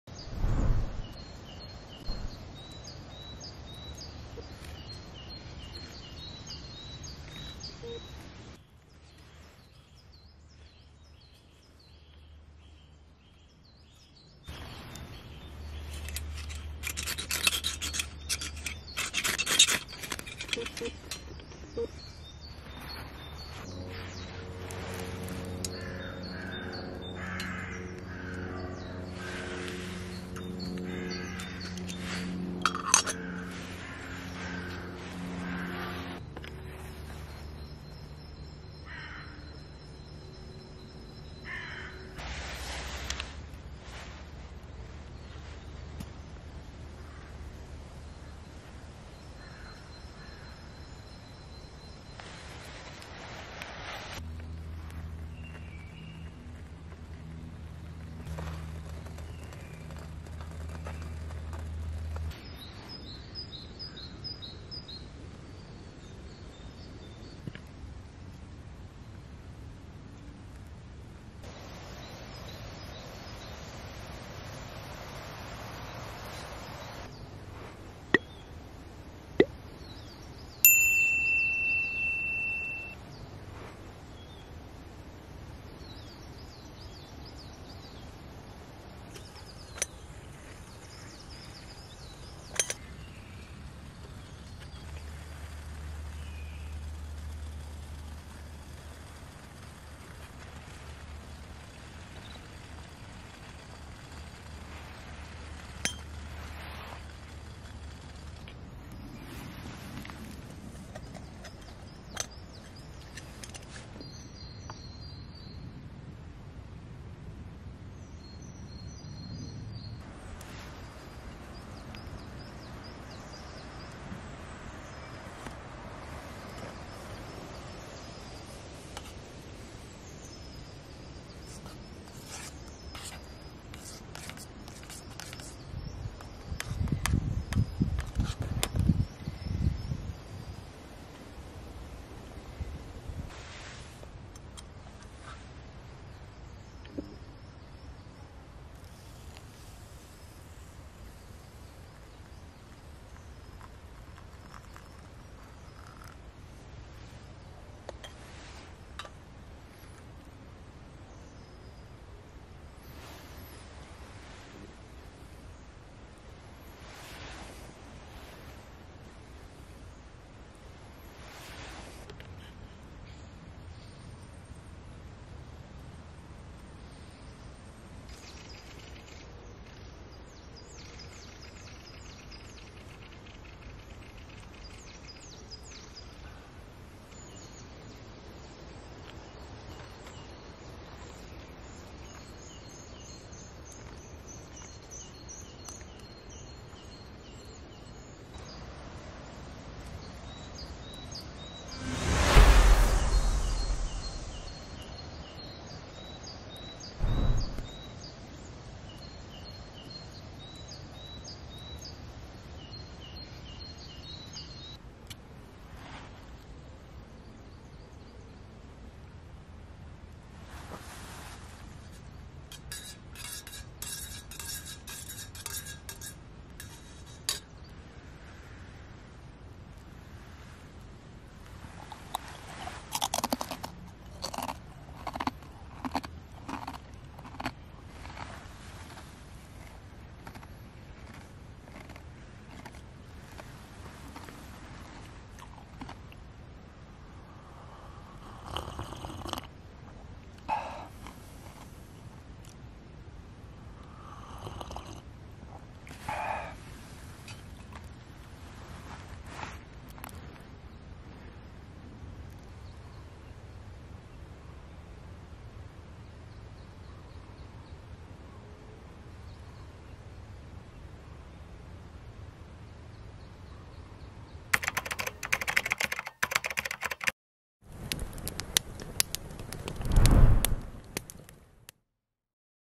Kaffee im Wald / Coffee in the Woods / Coffeelover
Hallo!
Der Frühling steht vor der Tür. Herrliche 15 Grad und Sonne pur. ☀️
Rucksack gepackt und ab in die Wald. Eine wunderbare Waldrunde und einen leckeren Kaffee im Wald. So sind sie die Coffeelover. ☕️
Der Wald war bereits voll von Vögel und ein herrliches Gezwitscher.
Also viel Spaß bei diesem kurzen Video für alle Kaffeeliebhaber.
 Geht raus und genießt den Frühling. 🌳☀️🌲

Stefan